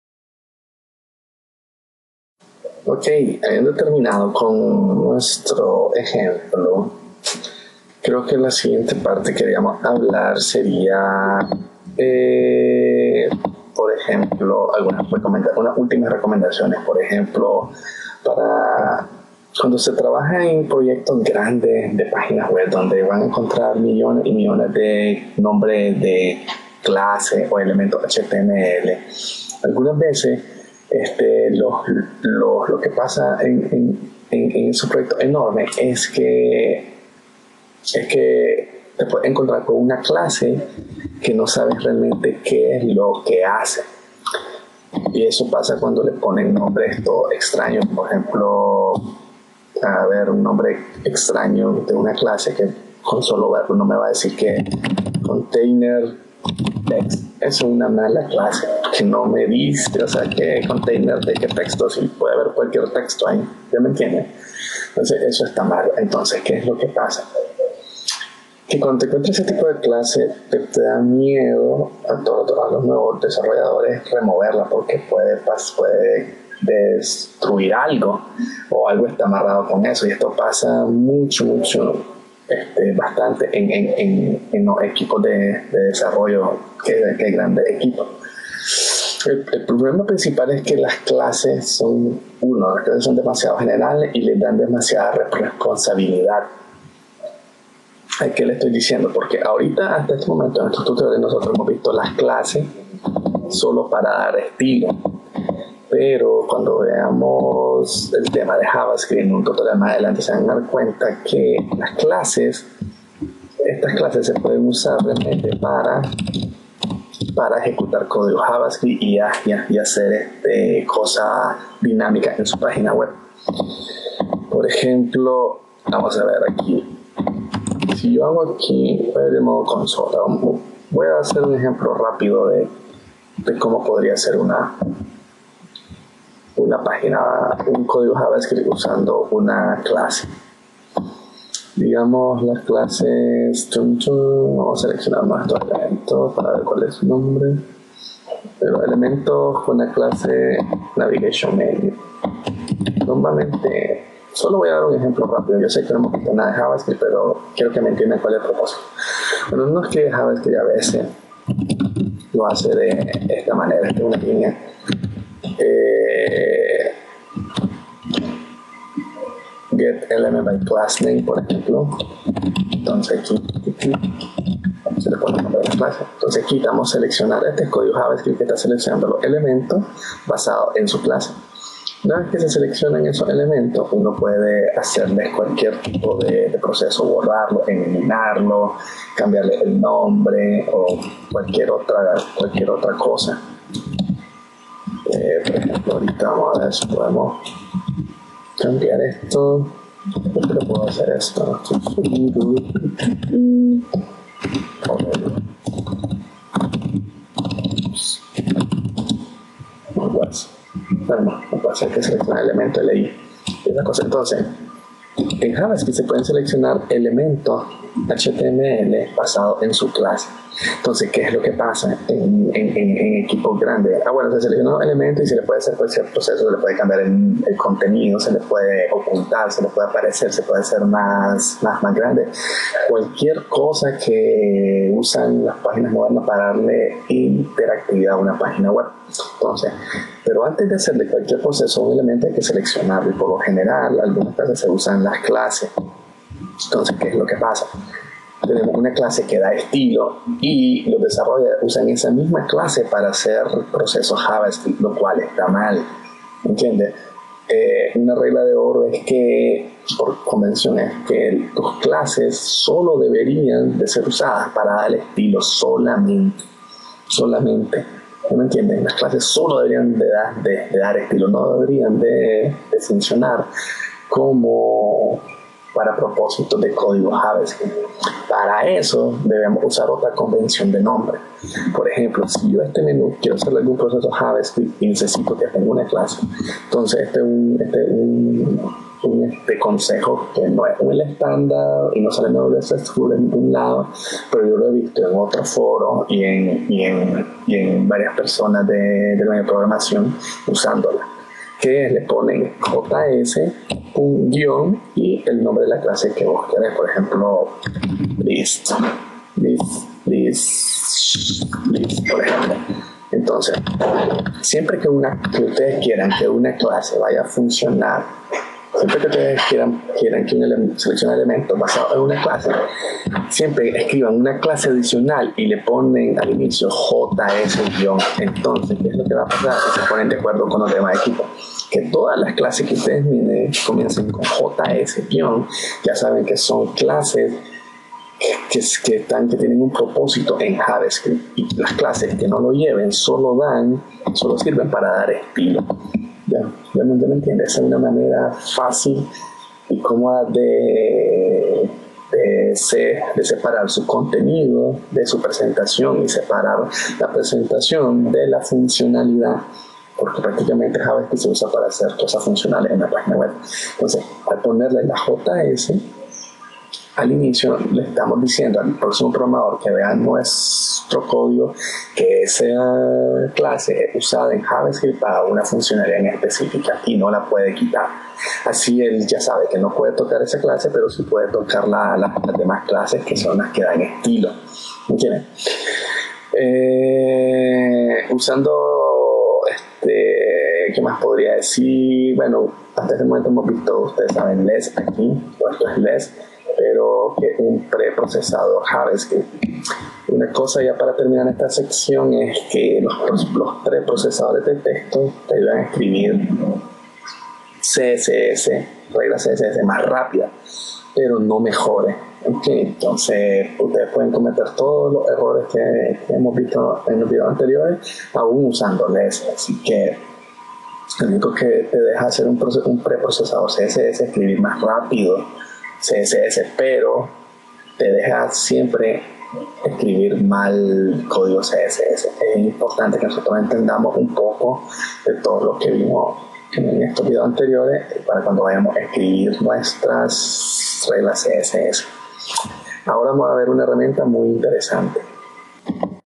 Curso CSS basico - Consejos para nombres de clases en CSS - 7
Escoger nombres para nuestra clases en css puede ser muy dificil, aprender algunos consejos en este video parte 7. Este vídeo es parte del curso de programación web basica Para mas cursos y el orden de los videos visita nuestra web